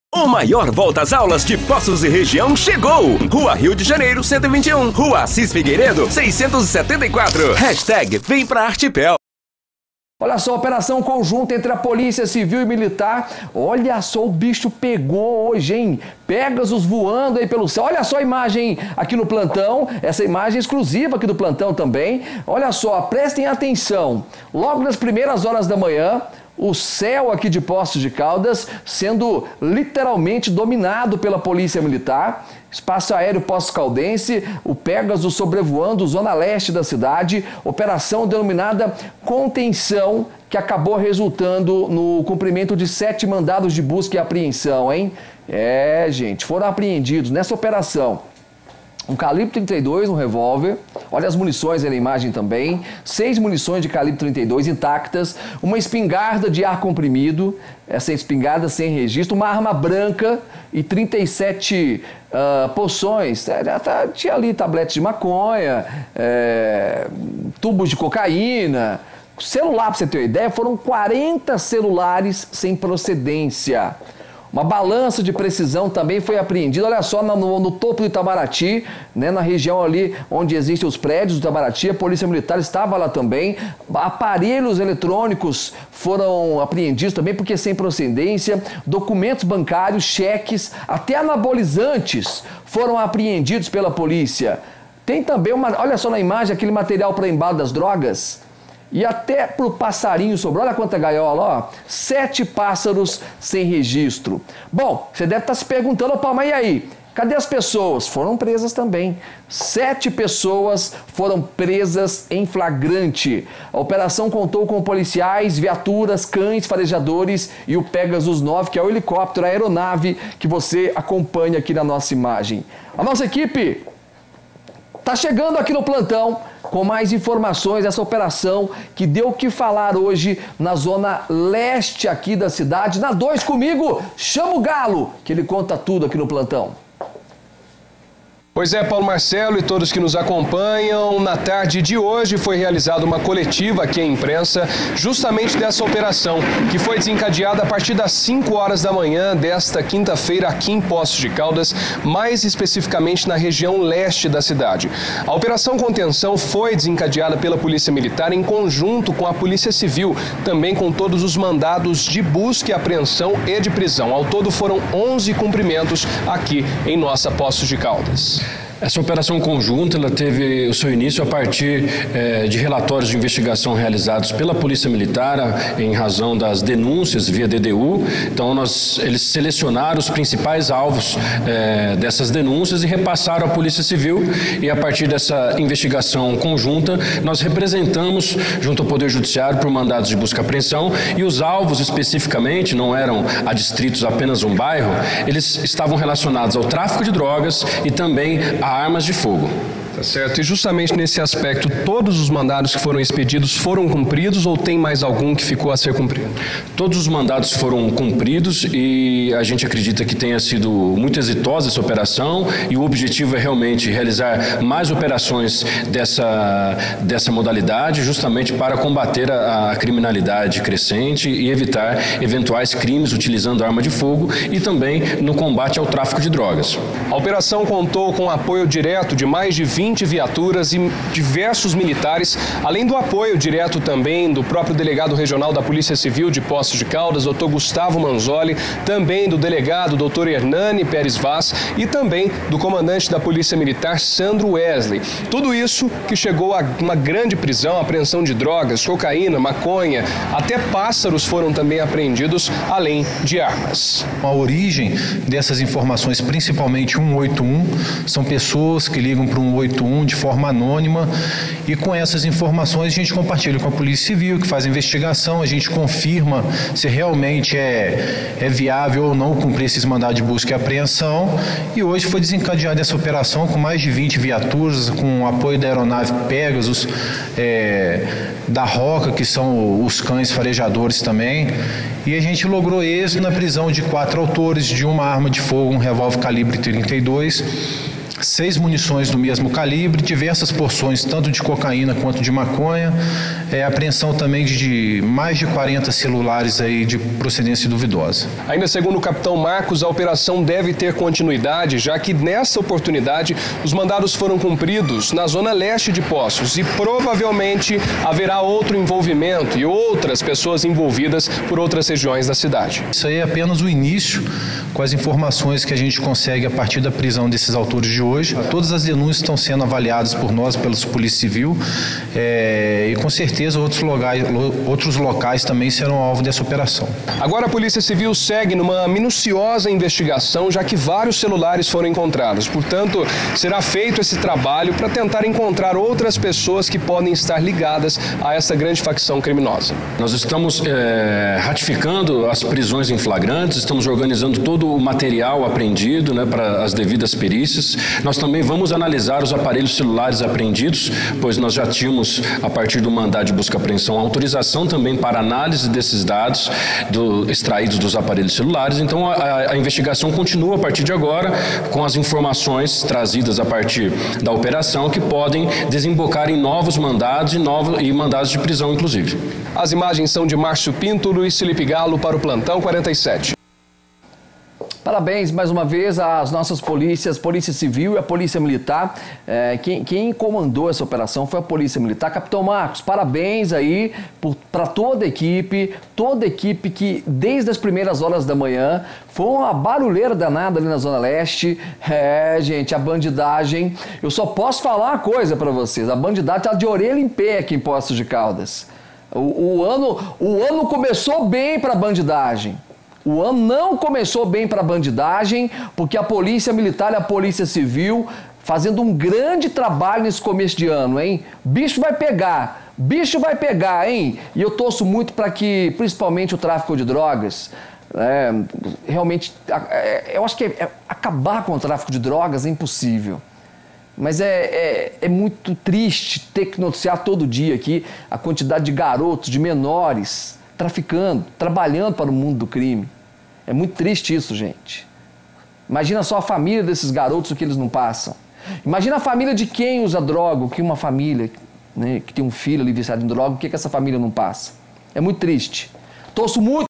OPERAÇÃO CONTENÇÃO
OPERAÇÃO CUMPRE 7 MANDADOS DE BUSCA E APREENSÃO

Operação Contenção, entre Polícias Civil e Militar cumpriu 7 mandados de busca e apreensão na Zona Leste de Poços de Caldas. Quatro pessoas foram presas em flagrante, a operação contou com cães e o helicóptero Pegasus-9.